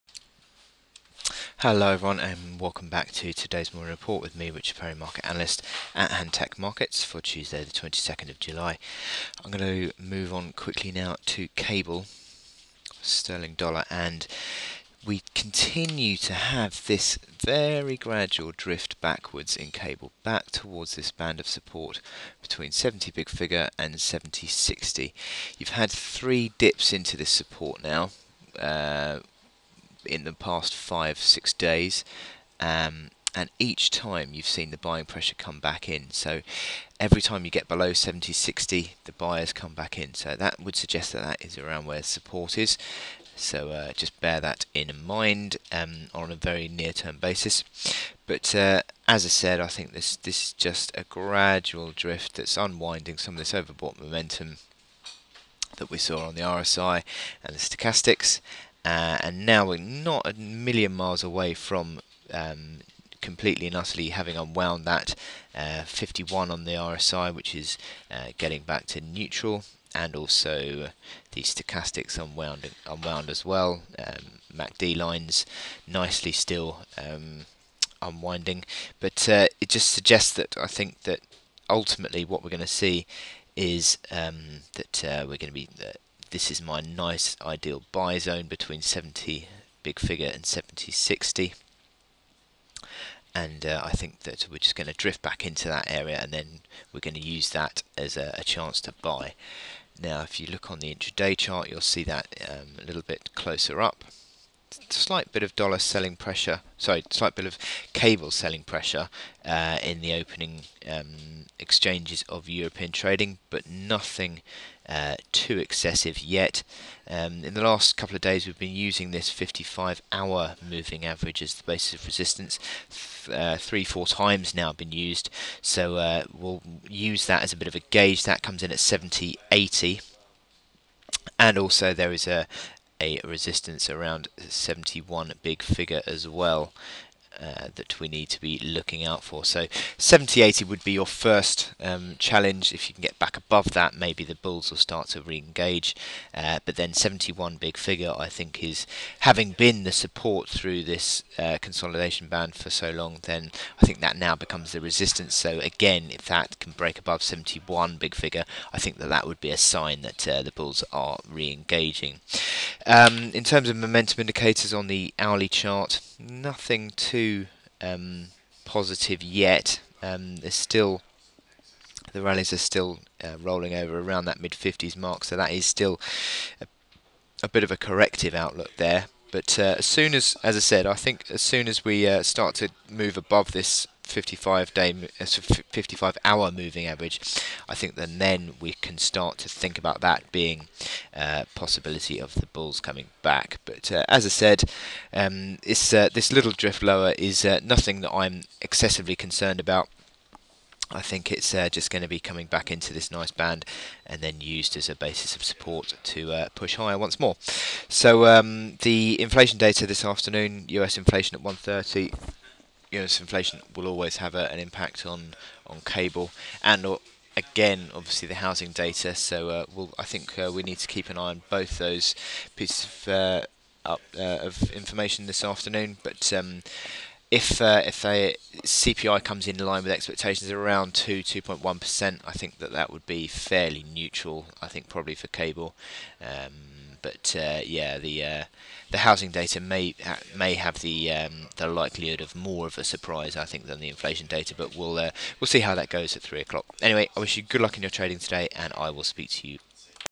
GBP/USD 22/07/2014
The consolidation drift shows little sign of abating yet, but the top of the support band $1.7000/$1.7060 which I believe is going to be a good "buy-zone" continues to be bought into. There have now been three dips below $1.7060 in the past five days and each time the buyers have returned. The momentum indicators have used this slight drift lower to unwind any overbought momentum and are now approaching levels that would be considered to be good  buying opportunities once more, with the RSI around 53. The intraday hourly chart may perhaps hold a clue for resumed upside. The falling 55 hour moving average (now $1.7080) has been the basis of resistance over the past three days and if Cable starts to trade above it , it would suggest that the near term corrective downside pressure is beginning to abate. The old support around $1.7100 also capped the rally from yesterday so would also be seen as a ceiling. A break back above would re-open $1.7150 and then the high again at $1.7191. There is minor support at $1.7034.